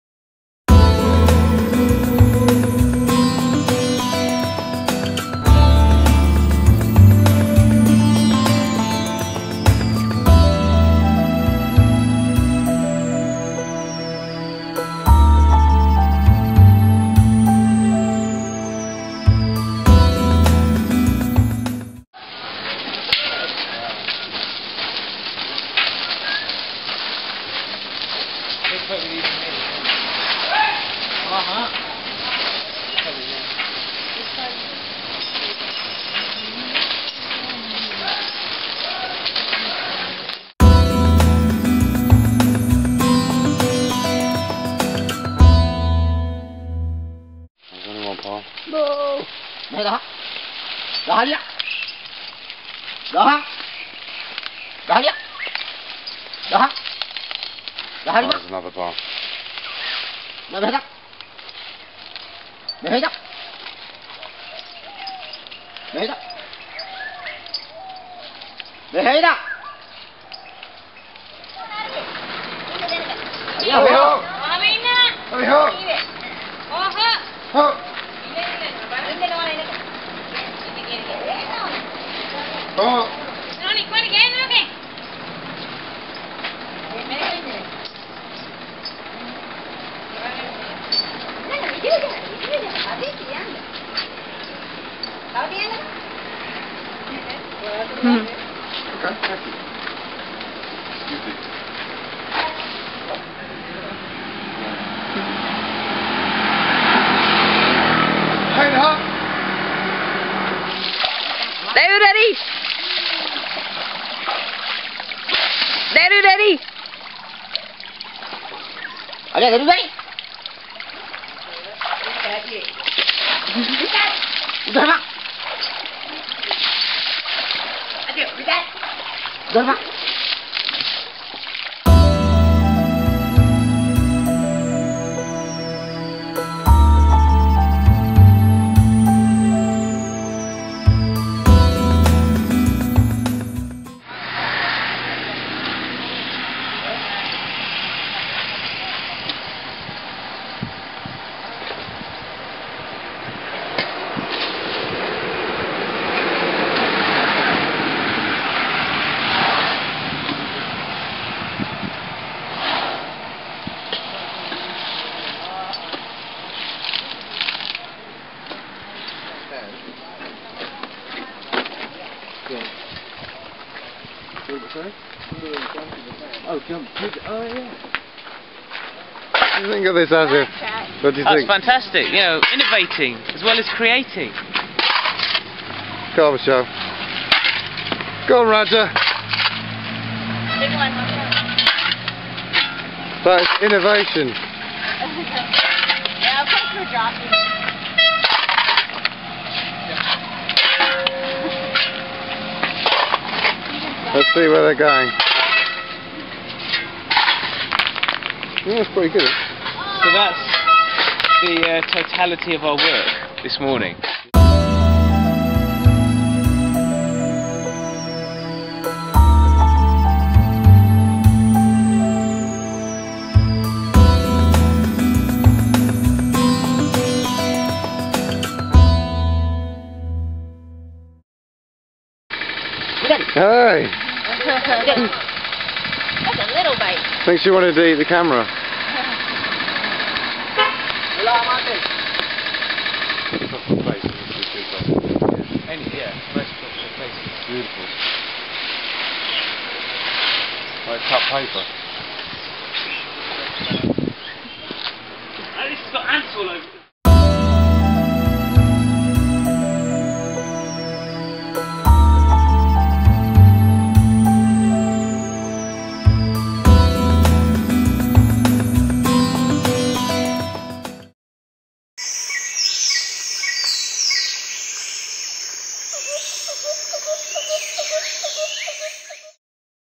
Volunteering with endangered Asian Elephants
A volunteer's experience working with endangered Asian Elephants at an elephant sanctuary in Kegalle, Sri Lanka.